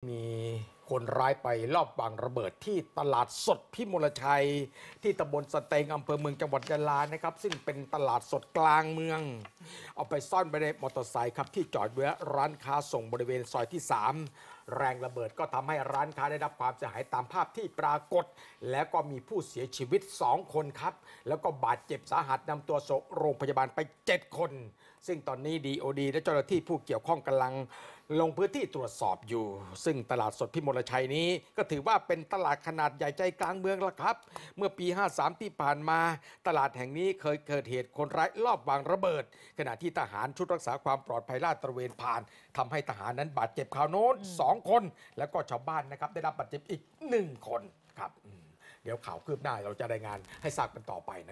ลอบบึ้มหน้าตลาดสดพิมลชัย เมืองยะลา ตาย 2 เจ็บ 7
เมื่อช่วงเช้าวันนี้ สภ.เมืองยะลา ได้รับแจ้งเหตุมีคนร้ายไม่ทราบจำนวน ลอบวางระเบิดหน้าตลาดสดพิมลชัย บริเวณร้านค้าส่ง ซอย 3 ต.สะเตง อ.เมืองยะลา จ.ยะลา เบื้องต้นมีผู้เสียชีวิต 2 ราย ได้รับบาดเจ็บ 7 ราย เจ้าหน้าที่นำตัวส่ง รพ.ศูนย์ยะลา รายละเอียดเพิ่มเติมอยู่ระหว่างการสืบสวนสอบสวนและตรวจสอบ

1.สารคดีเฉลิมพระเกียรติ
2.พระราชกรณียกิจ-พระมหากรุณาธิคุณ
3.คนไทยหัวใจ♥รักในหลวง